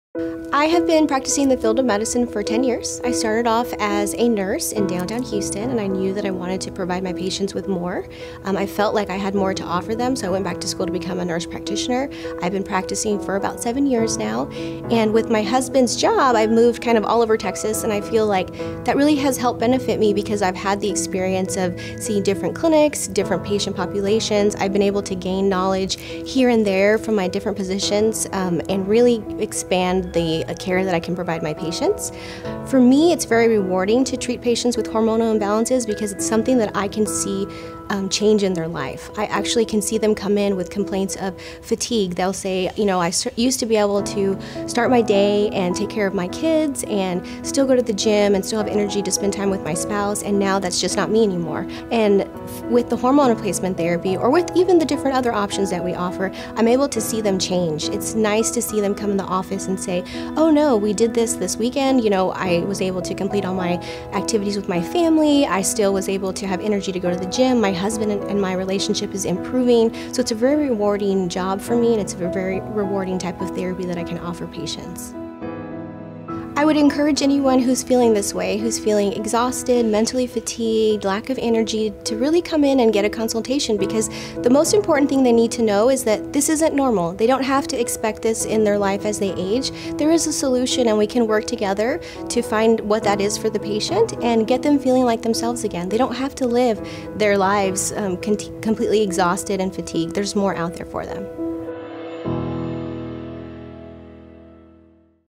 Overcoming Fatigue at Sandstone Family Medicine
Dr. Amanda talks about her career and how she approaches complaints of fatigue, exhaustion or lack of energy.